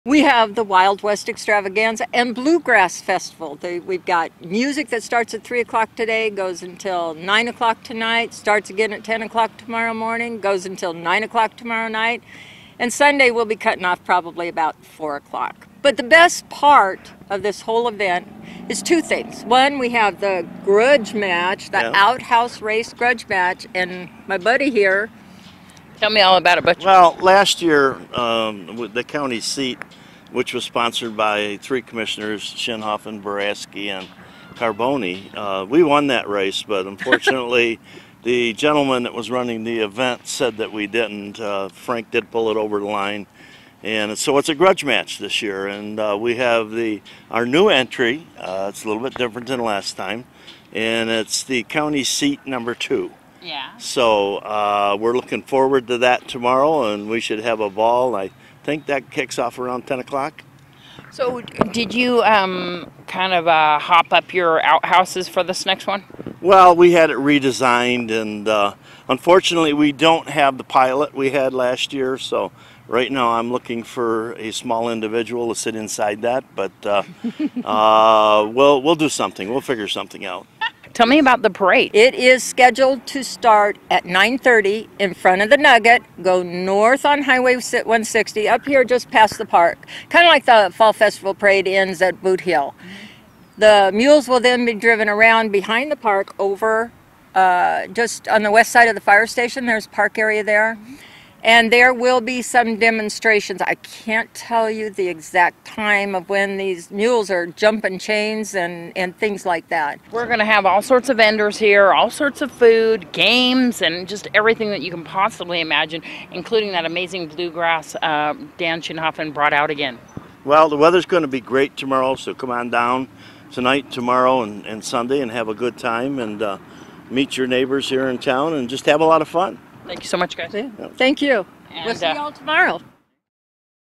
05/01/2015 Wild West Extravaganza & Bluegrass Festival
The Wild West Extravaganza and Blue Grass Festival is happening right now at Petrack Park.